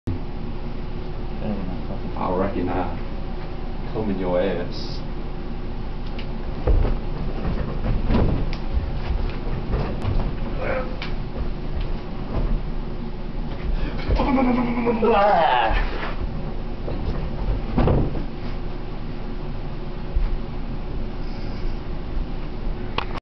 jumping off beds
jumping from bed to bed at the hotel in vegas